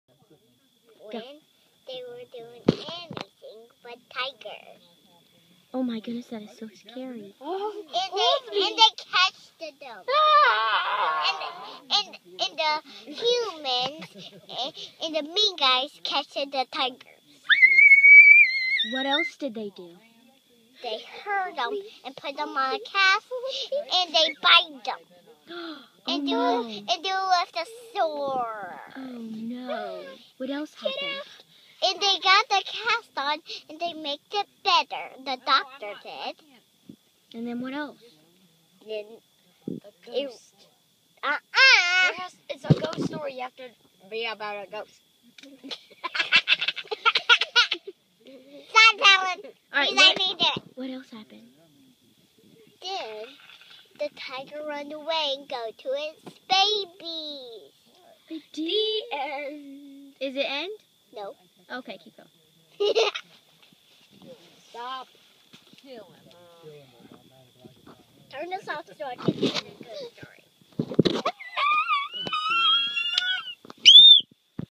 Lilly Joy fireside ghost story
4 year old Lilly's ghost story around the camp fire. Be scared